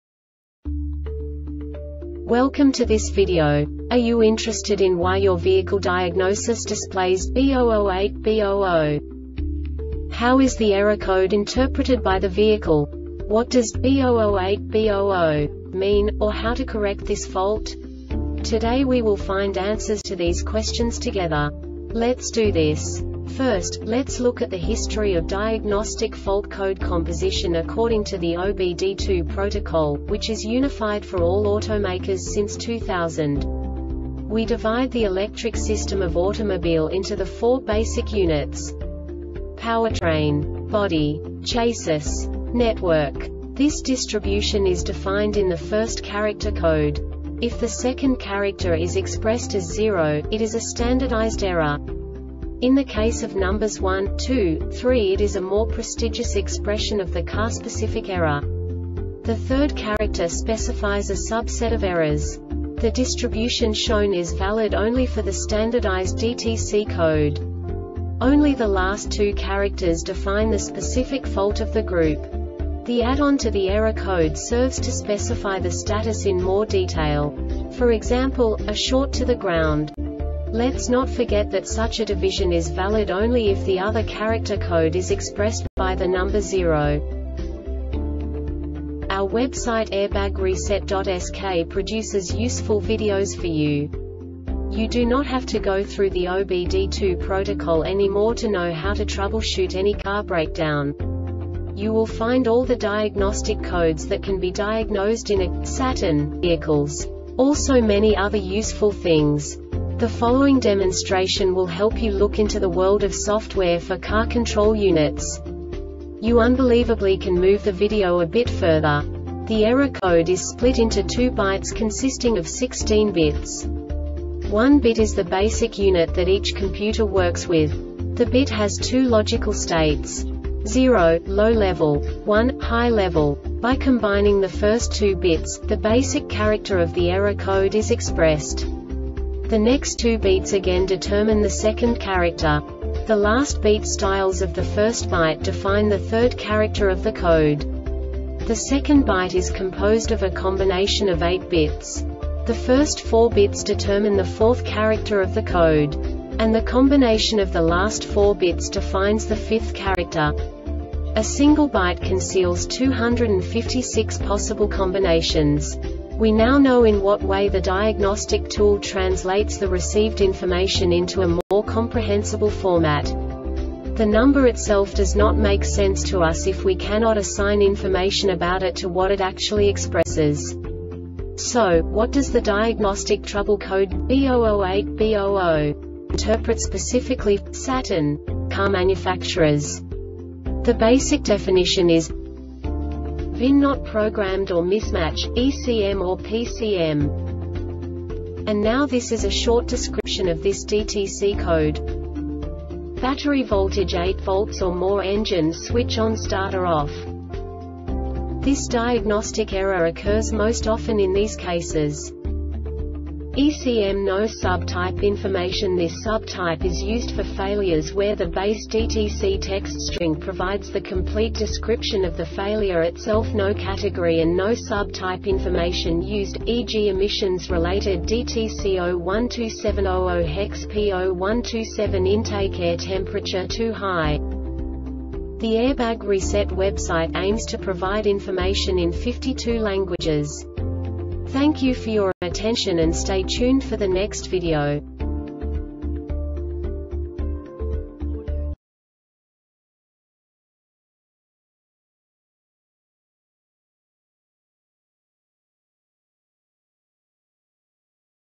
DTC Saturn B008B-00 Short Explanation
Contents:
0:21 Basic DTC analysis according to OBD2 protocol standard.
1:48 Insight into programming
2:58 A diagnostically understandable description of the Diagnostic Trouble Code
3:05 Basic error definition

Make:
Saturn
Code:
B008B 00
Definition:
VIN not Programmed or Mismatch - ECMorPCM
Description:
Battery voltage: 8 V or more Engine switch: ON Starter: OFF
Cause:
ECM
Failure Type: